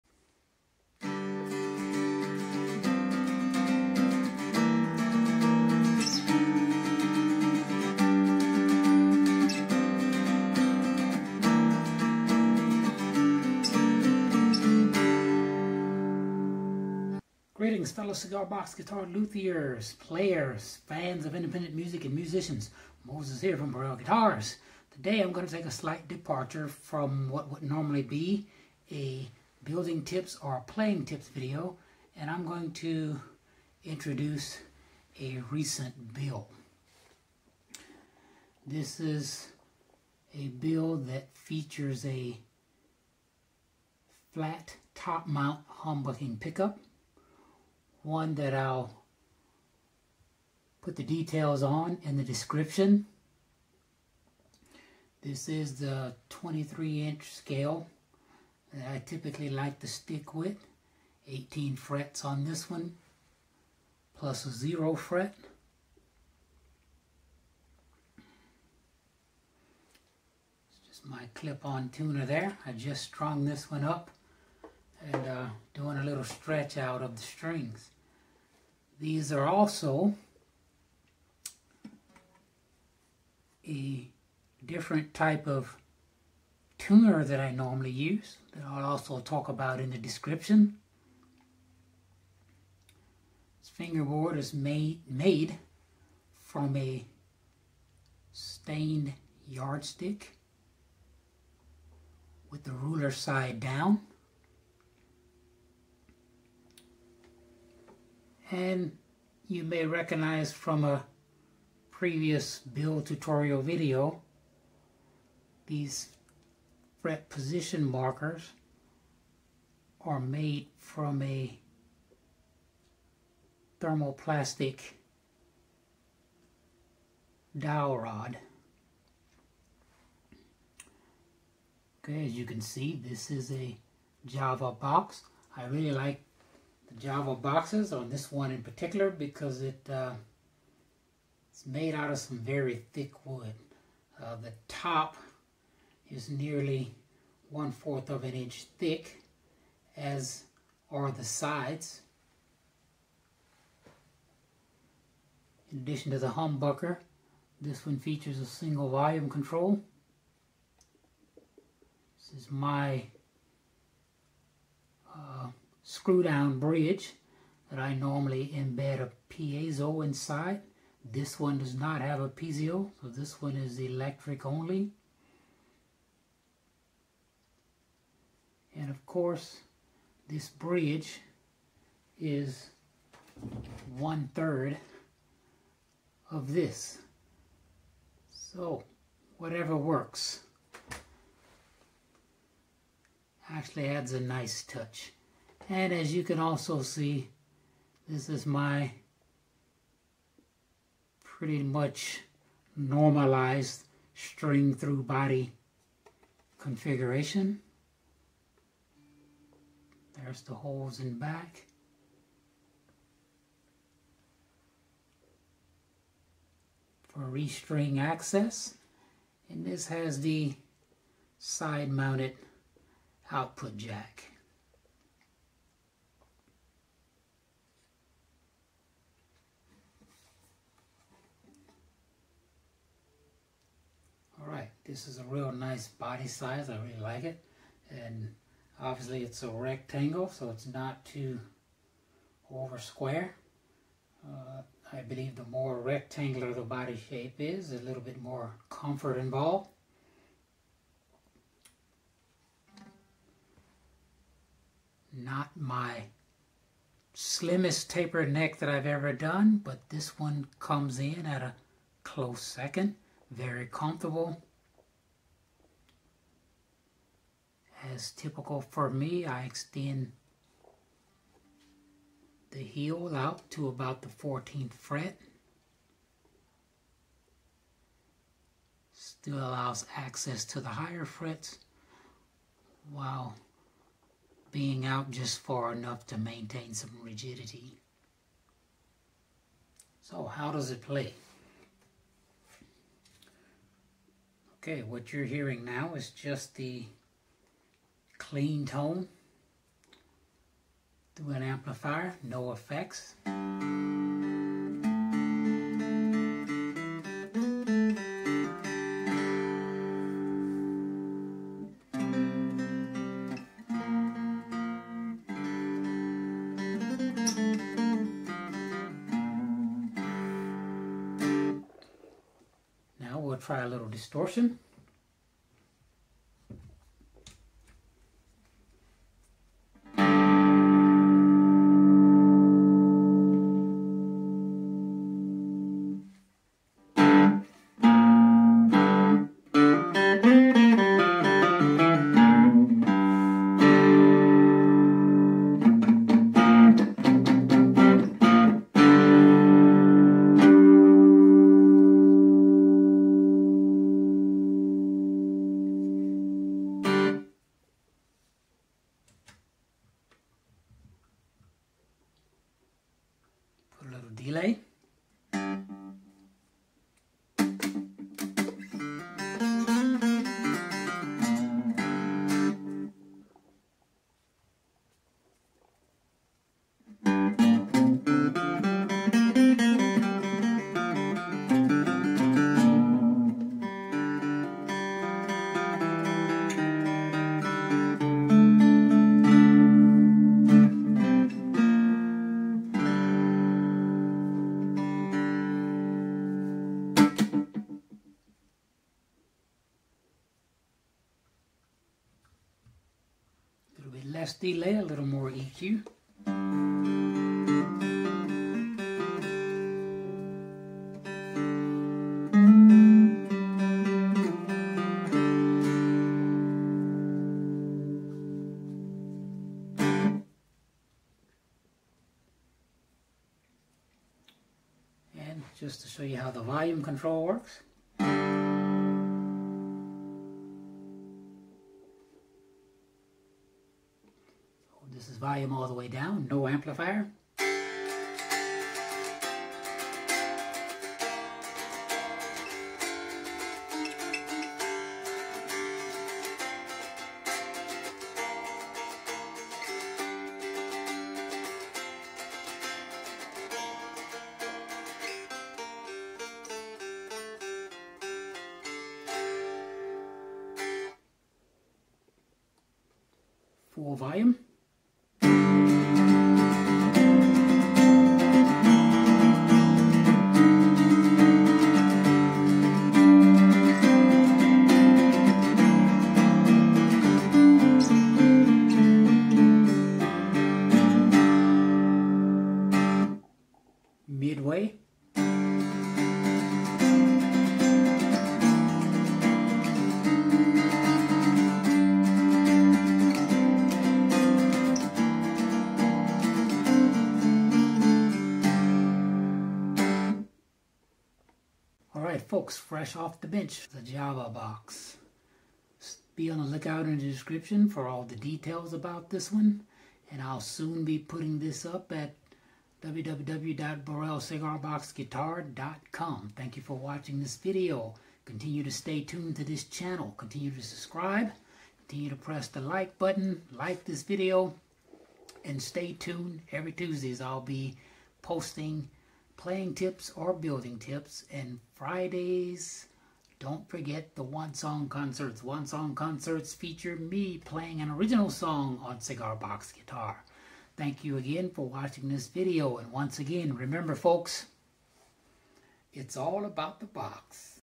Introducing the Java Box
This video is a demonstration of my latest CBG build.

Burrell CBG #19-21
Box Dimensions: 10" x 7" x 2"
Box Type/Cigar Brand: Java matte black finish
Tuners: MGB sealed chrome
Neck: poplar, polyurethane coated
String Guides: 7mm mini screw eyelets
Bridge: four-hole screw-down, poplar, routed for piezo and ground wire
Saddle: 1/4-inch turnbuckle eye bolt
Fretboard: spruce yardstick, stained
Electronics: MGB MIni Box Bucker Pickup 2.0 w/ stamped aluminum cover, 1/4" mono jack, 250k volume pot
Scale Length: 23 inches
Number of Frets: 18, medium nickel
Nut: jumbo (0) fret
Fret Position Markers: white 1/4" Acetal dowel rod from Grainger